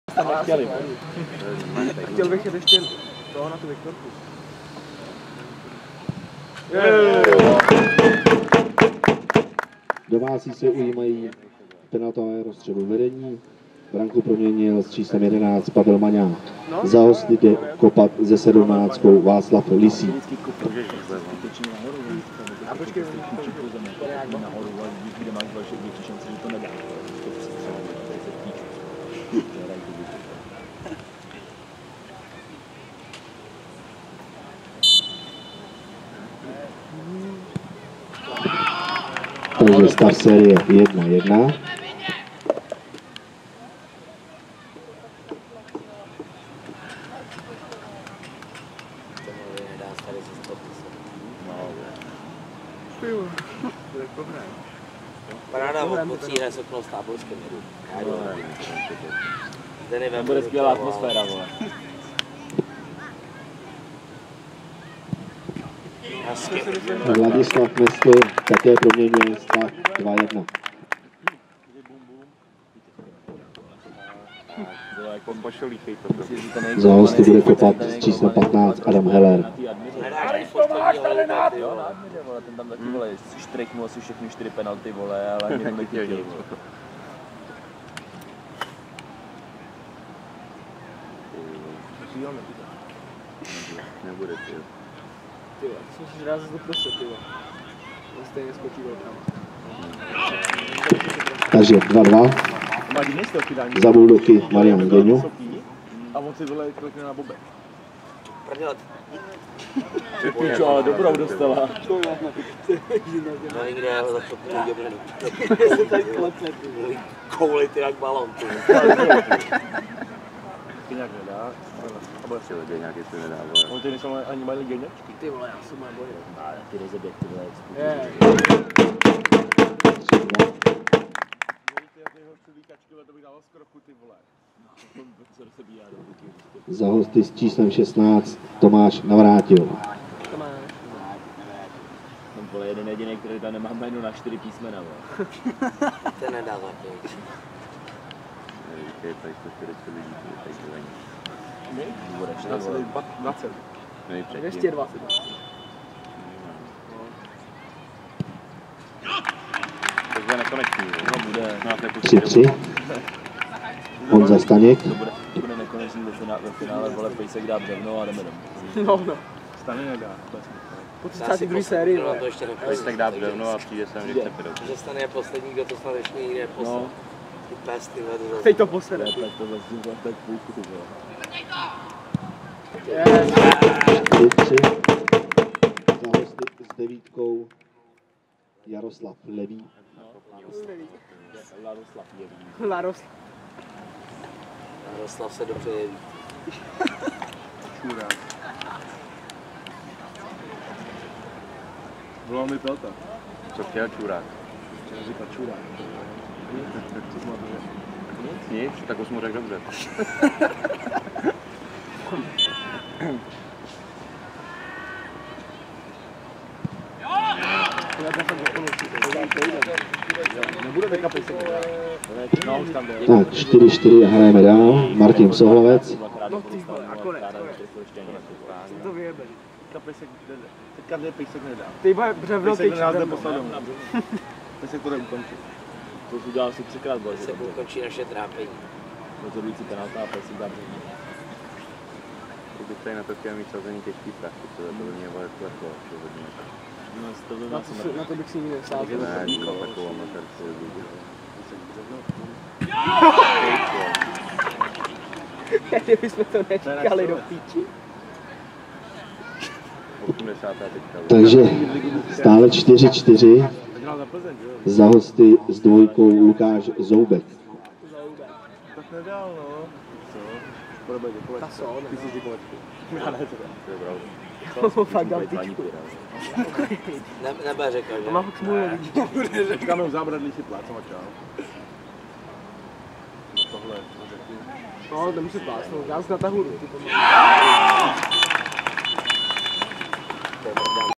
FC Slavia Karlovy Vary 0:0 (4:5) SK Petřín Plzeň penalty
FC Slavia Karlovy Vary